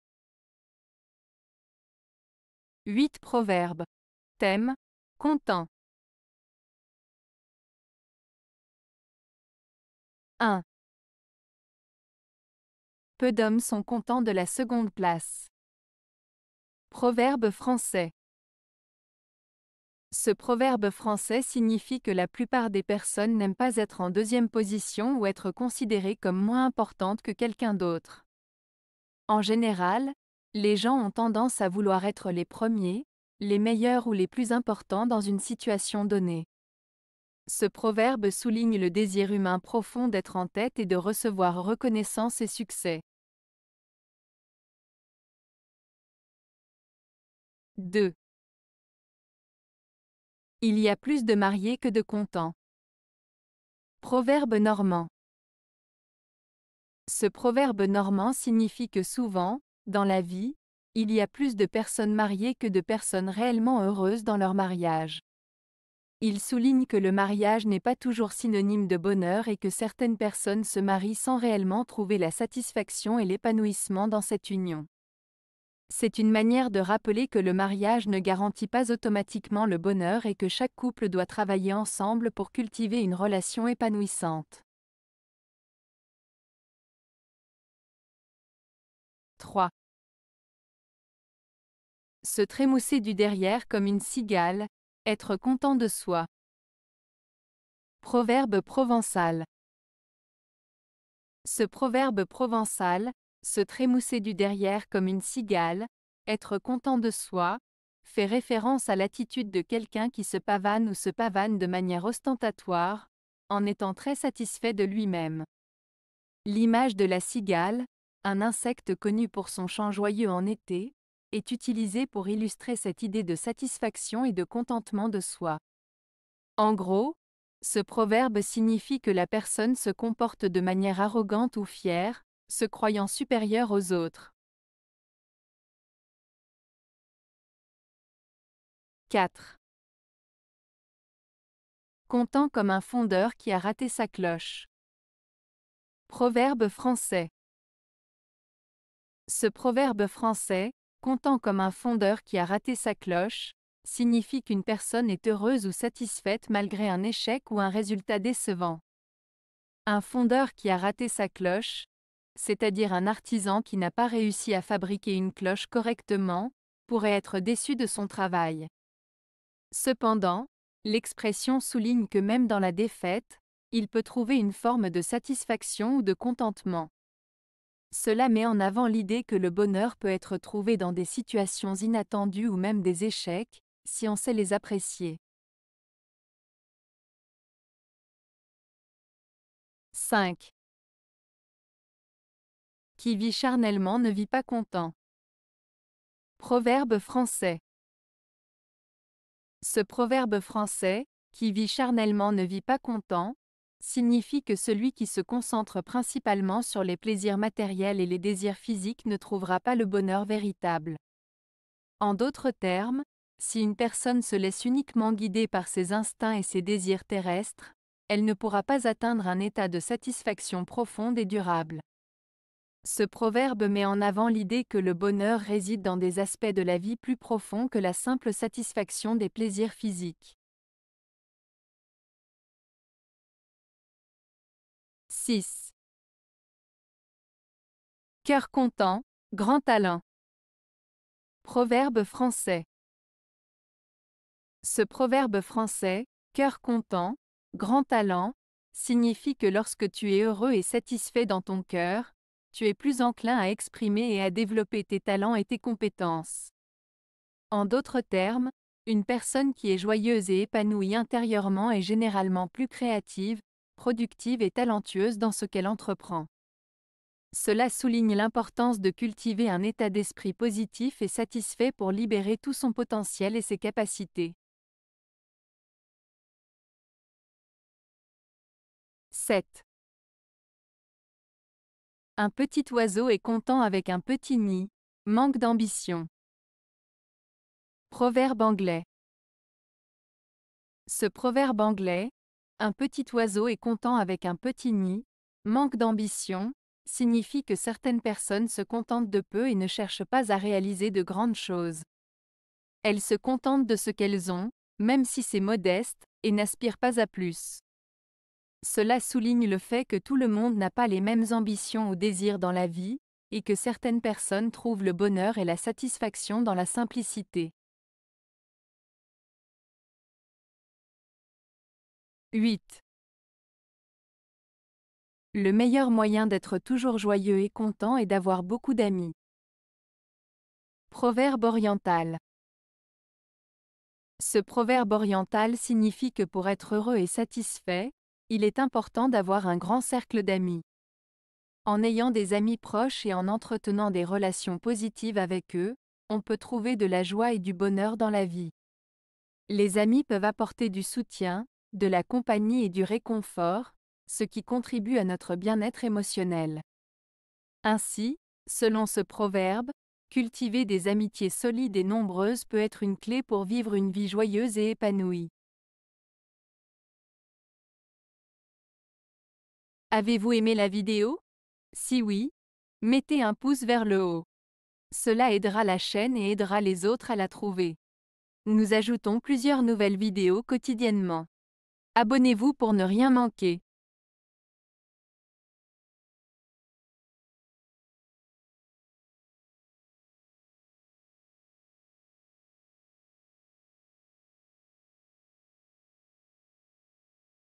Proverbes - Content (3 de 4)
00:00 Introduction
00:07 Proverbe Français - Peu d'hommes sont contents de la seconde place.
00:45 Proverbe Normand - Il y a plus de mariés que de contents.
01:26 Proverbe Provençal - Se trémousser du derrière comme une cigale (être content de soi).
02:16 Proverbe Français - Content comme un fondeur qui a raté sa cloche.
03:07 Proverbe Français - Qui vit charnellement ne vit pas content.
03:52 Proverbe Français - Cœur content, grand talent.
04:36 Proverbe Anglais - Un petit oiseau est content avec un petit nid (manque d'ambition).
05:19 Proverbe Oriental - Le meilleur moyen d'être toujours joyeux et content est d'avoir beaucoup d'amis.